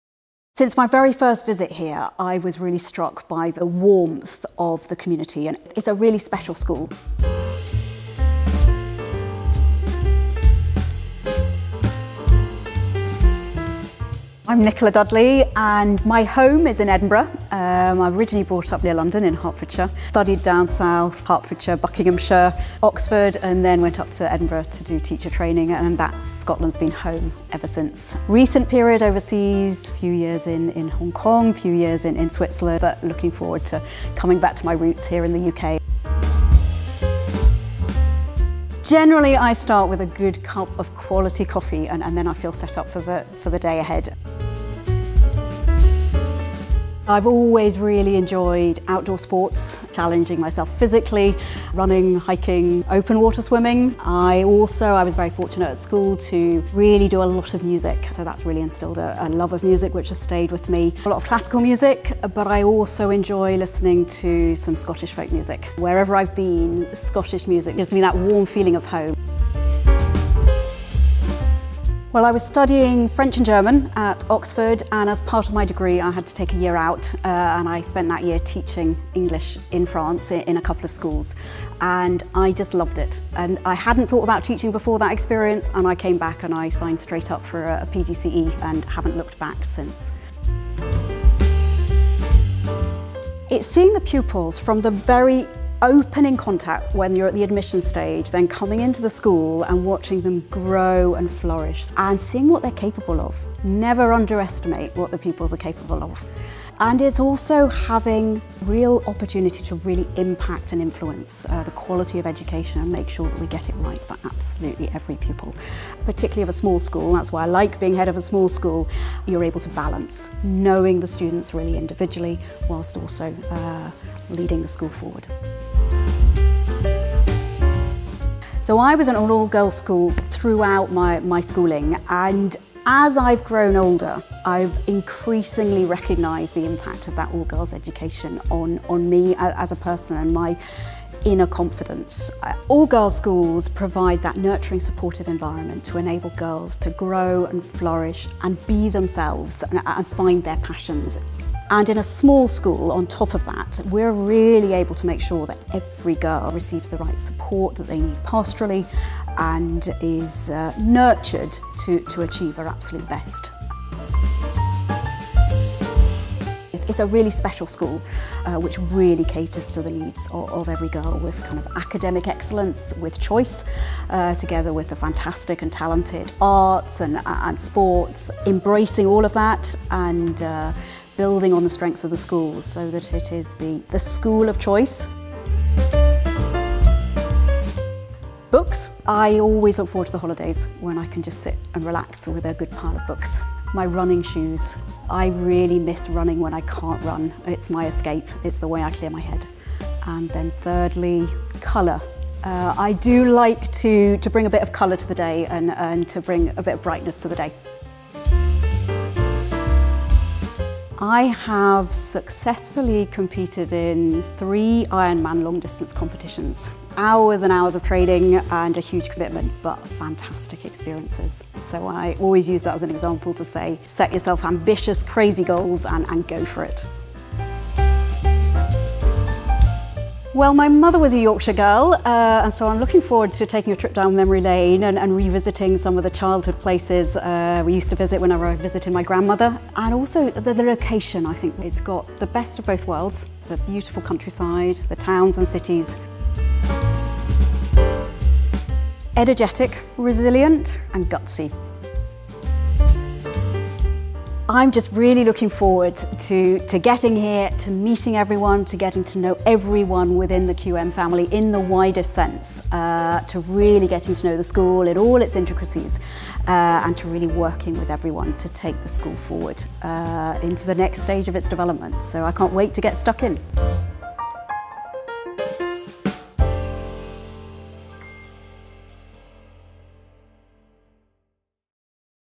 Nicola Dudley - New Head of Queen Margaret's School, York
QM is pleased to announce the appointment of Ms Nicola Dudley as its new Head.

Nicola has over 24 years' experience in independent education and is currently the Principal of Surval Montreux School for Girls in Switzerland. During a rigorous recruitment process which yielded an international field of candidates, Nicola stood out as a perfect match for QM because of her enthusiasm for girls’ education and her vast experience of working in boarding schools in the UK and overseas.

Terry Burt, Chairman of the Board of Governors said “We are delighted to have appointed a high-profile Head who is already a specialist in small-scale, girls-only education. When Nicola joins QM I am sure we will all appreciate her deeply-held conviction that girls' education is relevant and necessary in the modern world. Nicola will also bring an abundance of energy and drive which I am sure will percolate throughout the school.”

Nicola was educated at Wycombe Abbey, another leading girls school in Buckinghamshire, before gaining a first class honours degree from Jesus College, Oxford. A linguist by training, Nicola has taught at several leading independent schools before taking on a series of leadership roles at Lomond School (Deputy Head Teacher), Scottish Council of Independent Schools (Deputy Director), Malvern College International (Director of Education) and Malvern College Hong Kong (founding Deputy Head).

Nicola will take up her role during the Autumn Term. Current Head, Sue Baillie leaves QM at the end of this academic year; during the interregnum between Sue’s departure and Nicola’s arrival, Claire Sheard will step into the position of Acting Head from September.